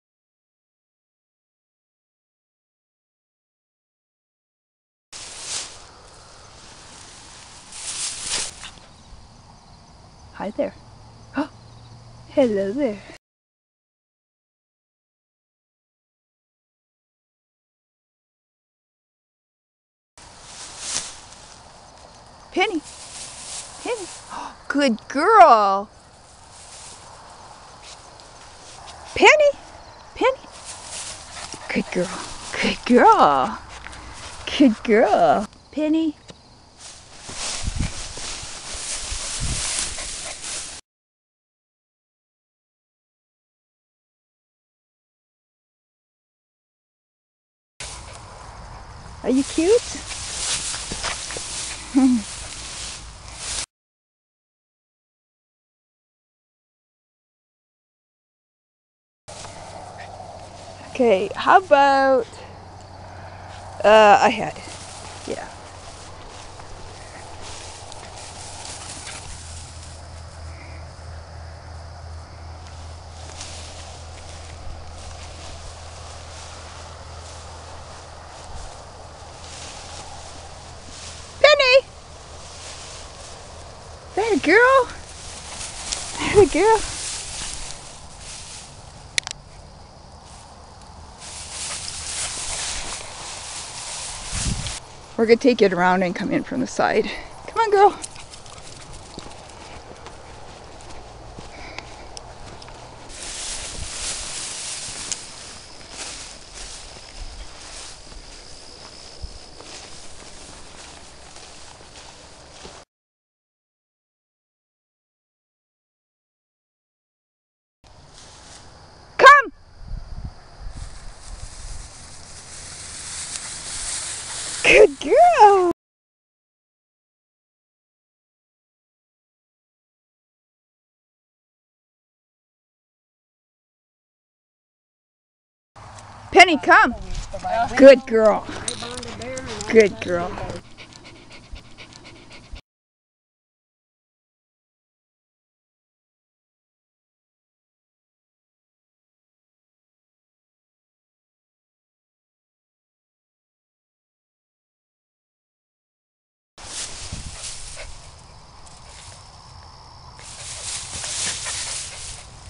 Practicing Come When Called With Penny The Brittany
Training Penny, a 9 week old American Brittany to come when called; utilizing a large fenced-in dog park.